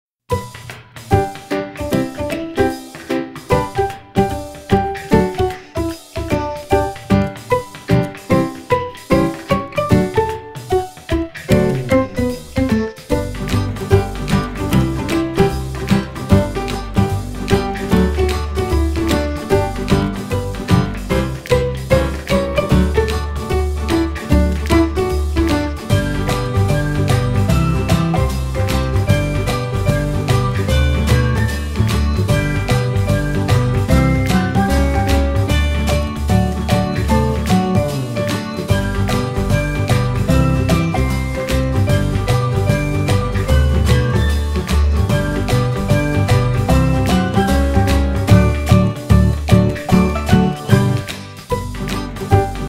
Mickey and Minnie Kiss Jigsaw Puzzle Time Lapse
This Mickey and Minnie puzzle is part of a multi-pack puzzle by Ceaco and is 750 pieces. This puzzle was really fun!